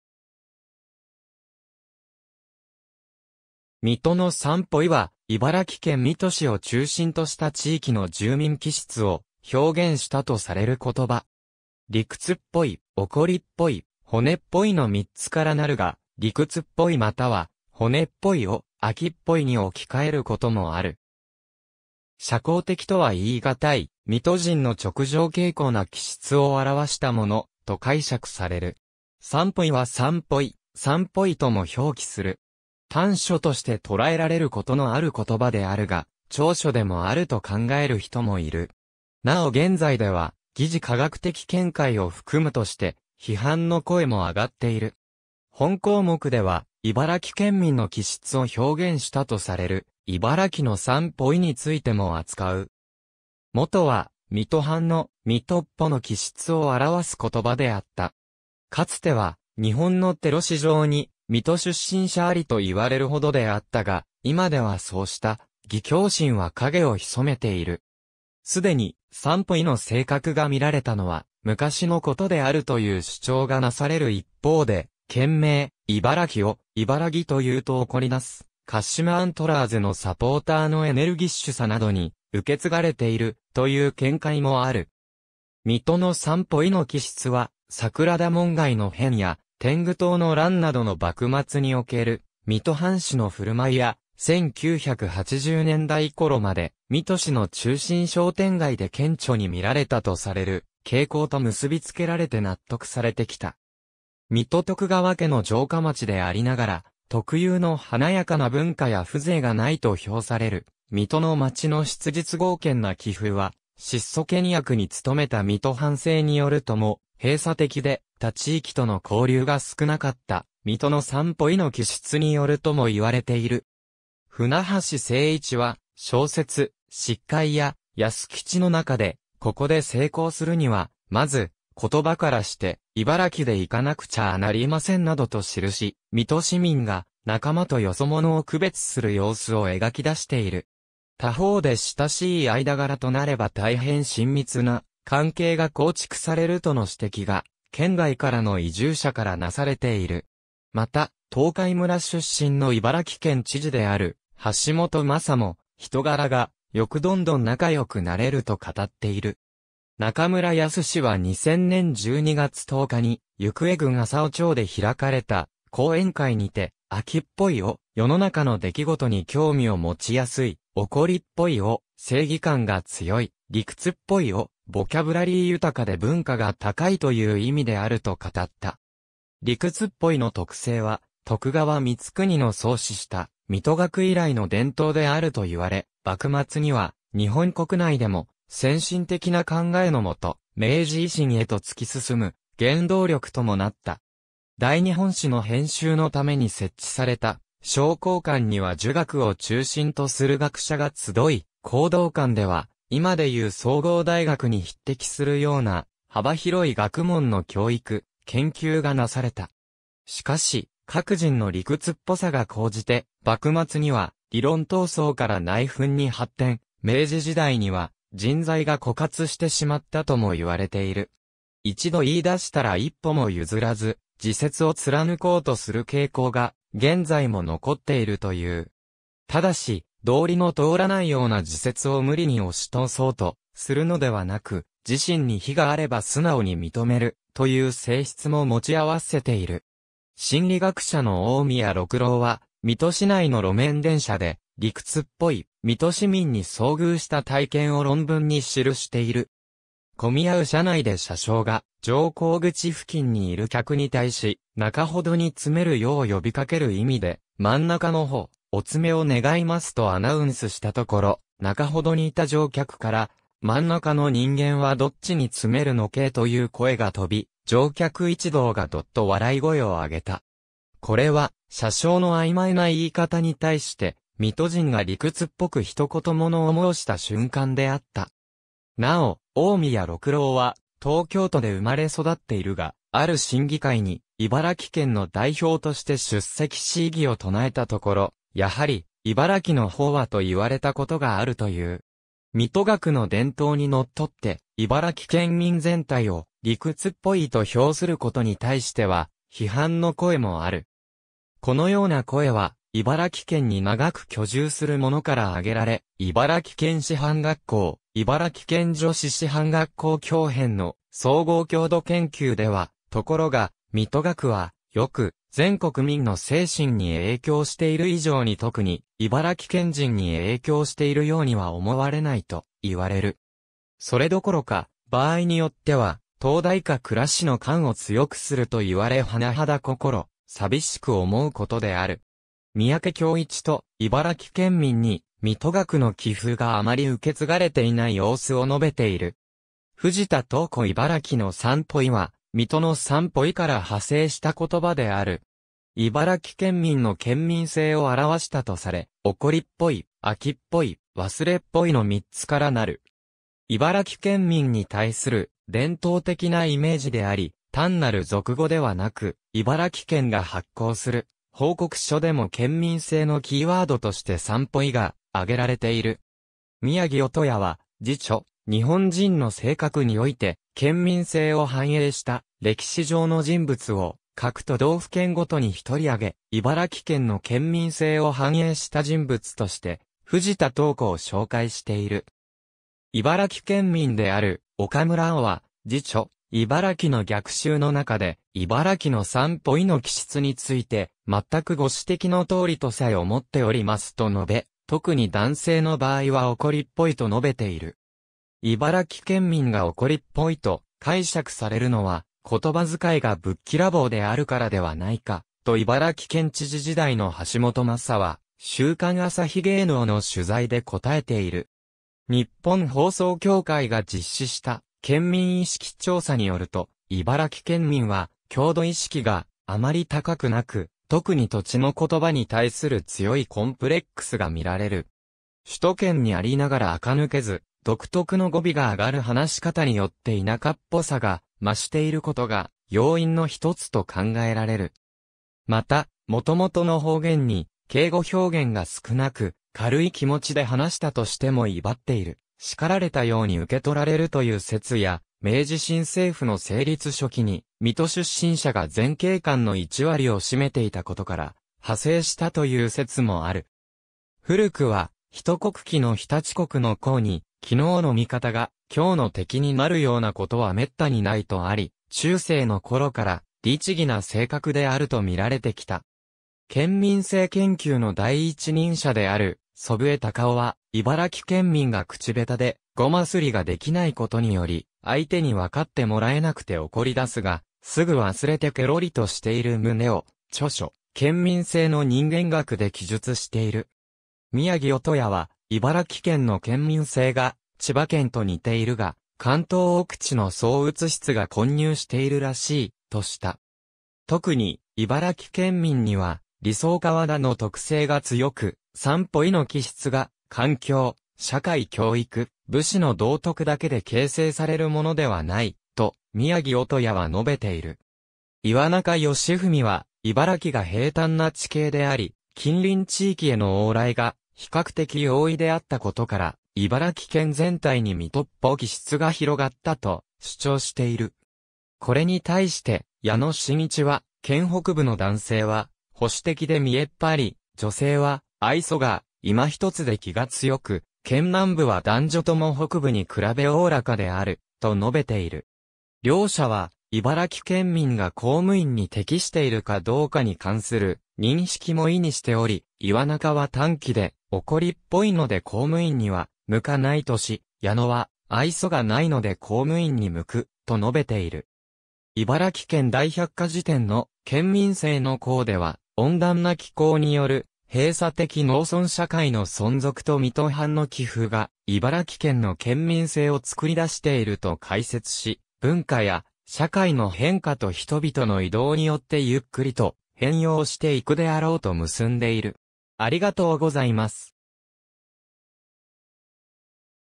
水戸の三ぽい
水戸の三ぽい（みとのさんぽい）は、茨城県水戸市を中心とした地域の住民気質を表現したとされる言葉。
理屈っぽい・怒りっぽい・骨っぽいの3つからなるが、「理屈っぽい」または「骨っぽい」を「飽きっぽい」に置き換えることもある。
社交的とは言い難い、水戸人の直情径行な気質を表したもの、と解釈される。
「三ぽい」は3ぽい、3ポイとも表記する。
短所として捉えられることのある言葉であるが、長所でもあると考える人もいる。
なお現在では疑似科学的見解を含むとして批判の声も上がっている。
本項目では、茨城県民の気質を表現したとされる、茨城の三ぽい（いばらきのさんぽい）についても扱う。
元は水戸藩の「水戸っぽ」の気質を表す言葉であった。
かつては「日本のテロ史上に水戸出身者あり」と言われるほどであったが、今ではそうした義侠心は影を潜めている。
既に「三ぽい」の性格が見られたのは昔のことであるという主張がなされる一方で、県名「茨城」を「いばらぎ」（正しくは「いばらき」）と言うと怒り出す、鹿島アントラーズのサポーターのエネルギッシュさなどに受け継がれている、という見解もある。
水戸の三ぽいの気質は、桜田門外の変や天狗党の乱などの幕末における水戸藩士の振る舞いや、1980年代頃まで水戸市の中心商店街で顕著に見られたとされる傾向と結び付けられて納得されてきた。
水戸徳川家の城下町でありながら特有の華やかな文化や風情がないと評される水戸の町の質実剛健な気風は、質素倹約に努めた水戸藩政によるとも、閉鎖的で他地域との交流が少なかった水戸の三ぽいの気質によるとも言われている。
舟橋聖一は小説『悉皆屋康吉』の中で、「ここで成功するには、先ず、言葉からして、茨城でいかなくちゃァなりません」などと記し、水戸市民が仲間とよそ者を区別する様子を描き出している。
他方で親しい間柄となれば大変親密な関係が構築されるとの指摘が県外からの移住者からなされている。
また、東海村出身の茨城県知事である橋本昌も、人柄がよくどんどん仲良くなれると語っている。
中村泰士は2000年（平成12年）12月10日に行方郡麻生町（現行方市）で開かれた講演会にて、「飽きっぽい」を「世の中の出来事に興味を持ちやすい」、「怒りっぽい」を「正義感が強い」、「理屈っぽい」を「ボキャブラリー豊かで文化が高い」という意味であると語った。
「理屈っぽい」の特性は、徳川光圀（水戸黄門）の創始した水戸学以来の伝統であると言われ、幕末には日本国内でも先進的な考えの下、明治維新へと突き進む原動力ともなった。
『大日本史』の編集のために設置された彰考館には儒学を中心とする学者が集い、弘道館では今でいう総合大学に匹敵するような幅広い学問の教育・研究がなされた。
しかし、各人の理屈っぽさが高じて、幕末には理論闘争から内紛に発展、明治時代には人材が枯渇してしまったとも言われている。
一度言い出したら一歩も譲らず、自説を貫こうとする傾向が現在も残っているという。
ただし、道理の通らないような自説を無理に押し通そうとするのではなく、自身に非があれば素直に認める、という性質も持ち合わせている。
心理学者の大宮録郎は、水戸市内の路面電車（茨城交通水浜線）で「理屈っぽい」水戸市民に遭遇した体験を論文に記している。
込み合う車内で車掌が乗降口付近にいる客に対し、中ほどに詰めるよう呼びかける意味で、「真ん中の方、おつめを願います」とアナウンスしたところ、中ほどにいた乗客から「真ん中の人間はどっちにつめるのけぇ」という声が飛び、乗客一同がどっと笑い声を上げた。
これは、車掌の曖昧な言い方に対して、水戸人が理屈っぽく一言物を申した瞬間であった。
なお、大宮録郎は東京都で生まれ育っているが、ある審議会に茨城県の代表として出席し異議を唱えたところ、「やはり茨城の方は」と言われたことがあるという。
水戸学の伝統に則って、茨城県民全体を「理屈っぽい」と評することに対しては、批判の声もある。
このような声は茨城県に長く居住する者から挙げられ、茨城県師範学校・茨城県女子師範学校共編の『総合郷土研究』（1937年）では、 ところが水戸学はよく、全国民の精神に影響して居る以上に特に茨城県人に影響して居るやうには思はれないといはれる。
それどころか場合によっては燈台下暗しの感を強くするといはれ甚だ心淋しく思ふことである。
— 三宅恭一 と、茨城県民に水戸学の気風があまり受け継がれていない様子を述べている。
藤田東湖 茨城の三ぽ...